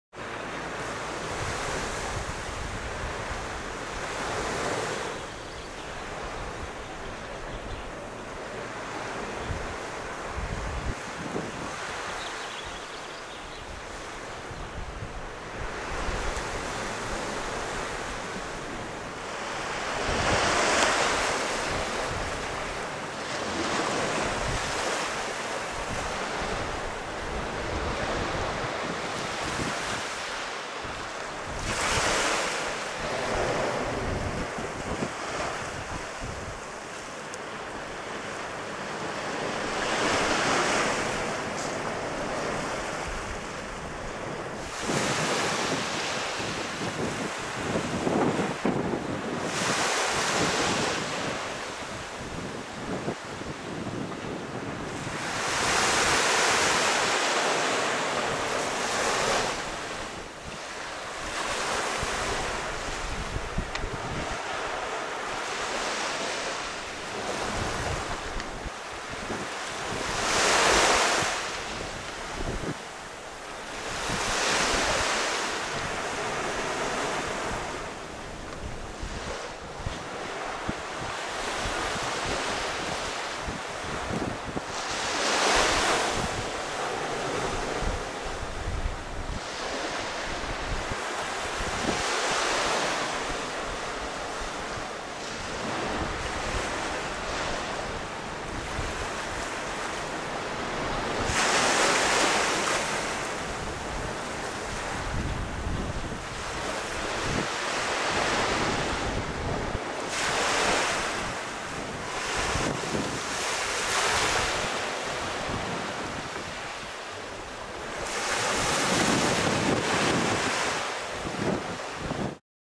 Cawsand - Cornwall 2011 720p
Cawsand in the Rame Peninsula, Cornwall 22nd June 2011